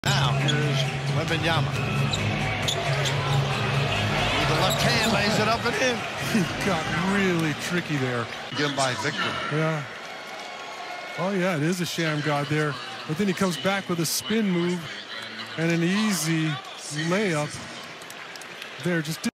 Victor Wembanyama shammgod unlocks easy layup
Shammgod unlocks easy layup by Victor Wembanyama

Leave a Like for more NBA videos!

Follow all my accounts